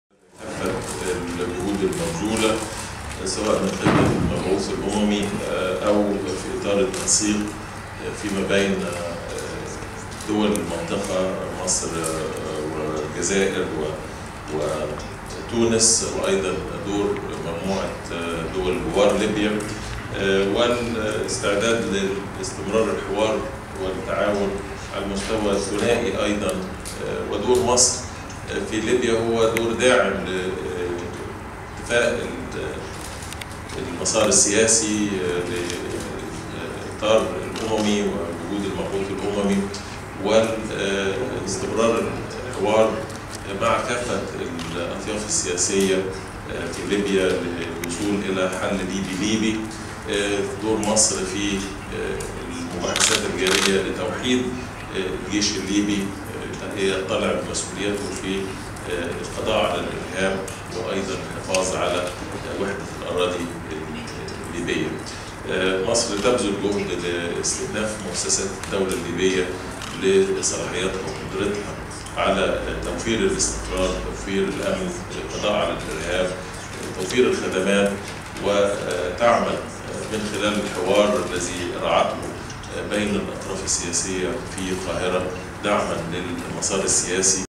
شكرى: لابد من حلول «ليبية - ليبية» تقضى على مخاطر الإرهاب
أكد السفير سامح شكرى، وزير الخارجية، أن موقف مصر ثابت تجاه الأزمة الليبية، مشددًا على دعم مصر لجيش ليبيا الوطنى، ولجميع مؤسسات الدولة فيها.